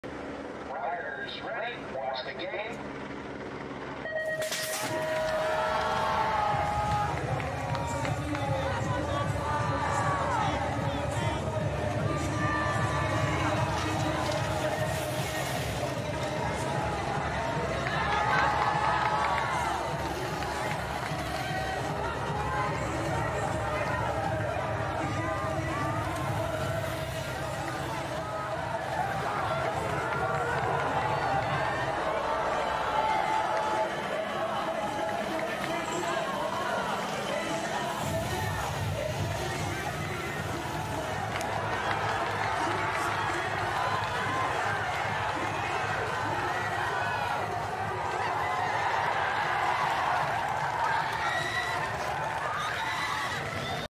Final Mariana Pajón Tokio 2021 (video completo)
Debate de la colombiana Mariana Pajon en la final de BMX Racing Femenino.
Medalla de plata con sabor a diamante 💎
Felicidades 100% Orgullo colombiano 🇨🇴